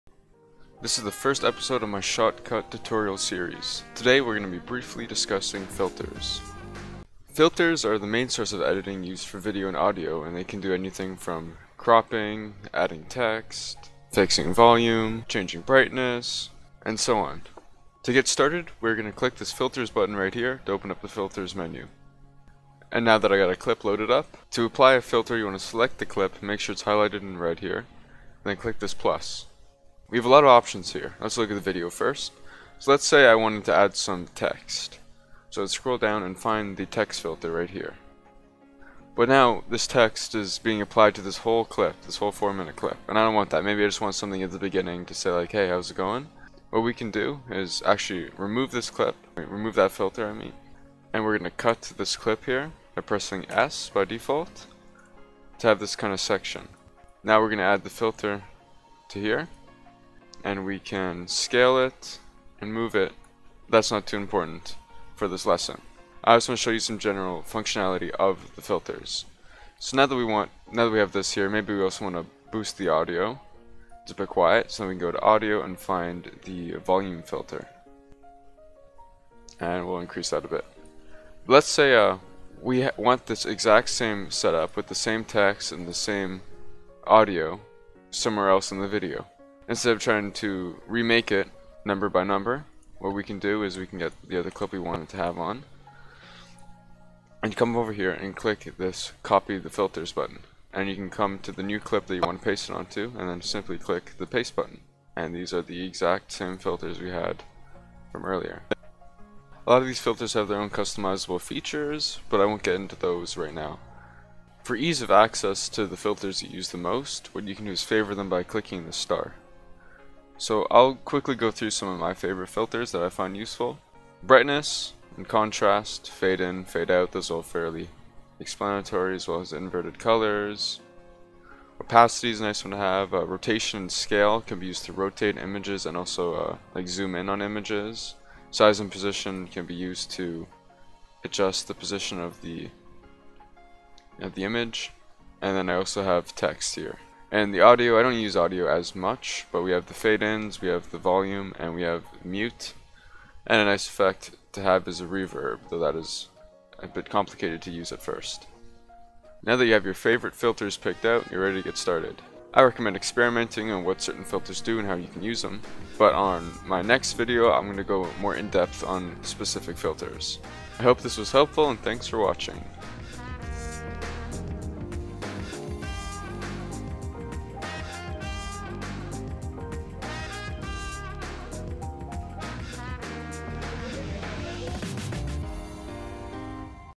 Shotcut; A beginners editing software. Intro to Filters (1)
Part one on a short serious about the free editing software Shotcut (v19.04.30). Here we discuss what filters are and how they work. Next episode we will discuss what certain filters do and how to use them.

If I'm still alive, i'll try my best to lend a helping hand.